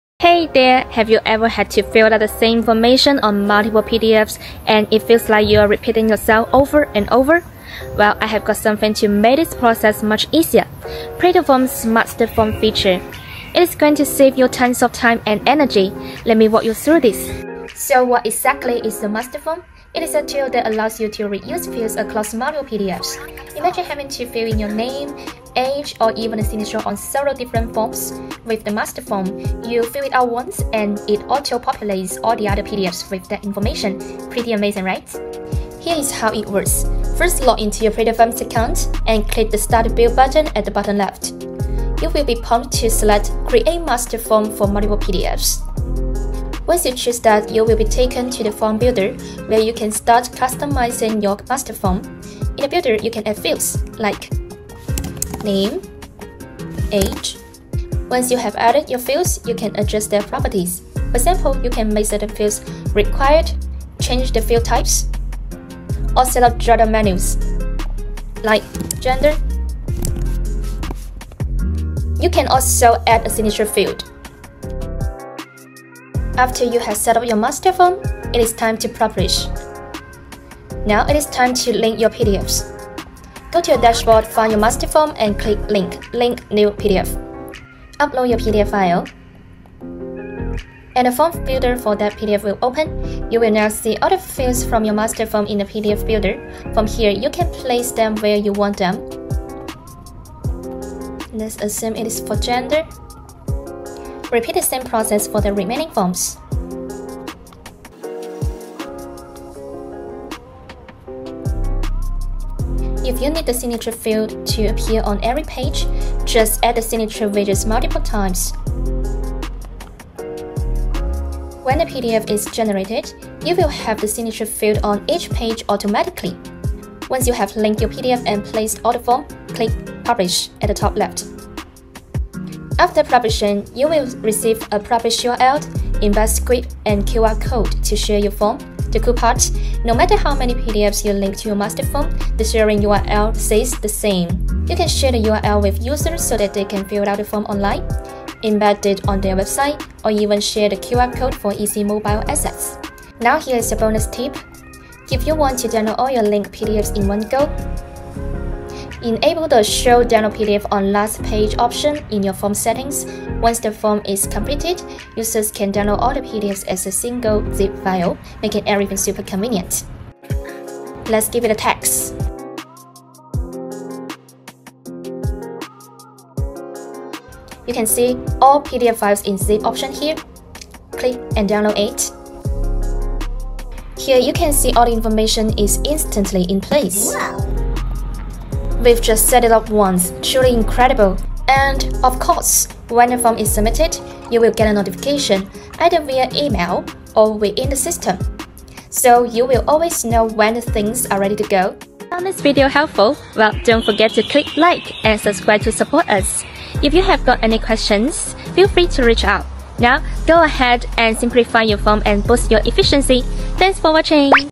Create Once, Auto-Populate Multiple PDFs! (Master Form Feature Demo - Save Time & Effort!)
Hey there! 🙋‍♀️ Are you tired of filling out the same details on every PDF form? Name, age, signature—repeated over and over again? Well, I’ve got a solution that’ll save you tons of time and energy: PlatoForms’ Master Form feature!

In this video, I’ll walk you through exactly how you can use the Master Form to simplify your form-filling process across multiple PDFs. Whether you’re handling forms for your business, team, or personal use, this tool makes repetitive form entries a thing of the past.

🔹 What is the Master Form?
🔹 How to build and customize your Master Form
🔹 How to link multiple PDFs to a single Master Form
🔹 Bonus tip: Download all PDFs in one go with a ZIP file
🔹 How to share your form easily with a public URL, embed code, or QR code

Step-by-step process:
 1. Log in to PlatoForms and select “Create Master Form for Multiple PDFs.”
 2. Add fields like Name, Age, Signature, and more. Customize each field for your needs.
 3. Link your PDFs and position your Master Form fields exactly where you want them.
 4. Publish your form, and share it with users using a Public URL, Embed Script, or QR Code.
 5. Download all the linked PDFs in one go—super convenient!

This feature is perfect for businesses, teams, or anyone looking to streamline their form management process and reduce repetitive work.

Have any questions? Drop them in the comments below or reach out to our support team!

Time Stamps:
0:00 – Introduction
0:17 – What is the Master Form?
0:35 – Setting up your Master Form
1:23 – Linking your PDFs
2:12 – Publishing & Sharing your form
2:23 – Bonus tip: Download all PDFs as a ZIP file
3:22 – Conclusion & Outro